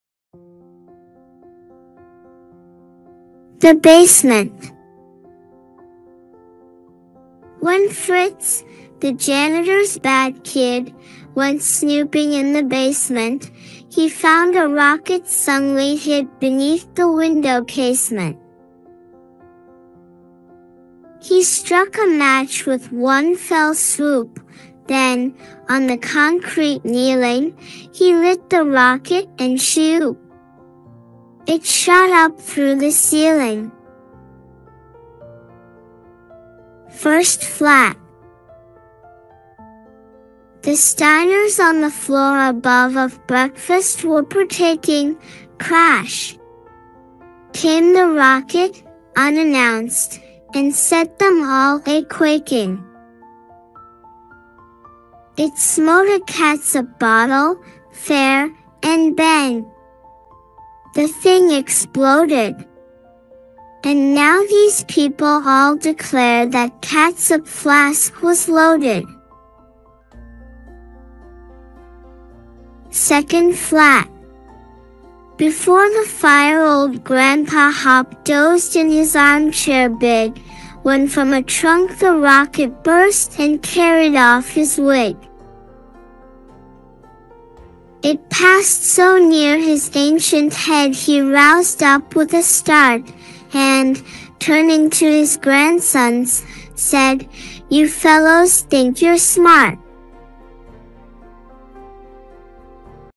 The ROCKET Book (1)
The ROCKET Book
by
PETER NEWELL HARPER & BROTHERS
NEW YORK

Content:
THE BASEMENT
When Fritz, the Janitor's bad kid,
Went snooping in the basement,
He found a rocket snugly hid
Beneath the window casement.

He struck a match with one fell swoop;
Then, on the concrete kneeling,
He lit the rocket and-she-oop!
It shot up through the ceiling.
FIRST FLAT
The Steiners on the floor above
Of breakfast were partaking
Crash! came the rocket, unannounced,
And set them all a-quaking!

It smote a catsup bottle, fair,
And bang! the thing exploded!
And now these people all declare
That catsup flask was loaded.
SECOND FLAT
Before the fire old Grandpa Hopp
Dozed in his arm-chair big,
When from a trunk the rocket burst
And carried off his wig!

It passed so near his ancient head
He roused up with a start,
And, turning to his grandsons, said,
" You fellows think you're smart!"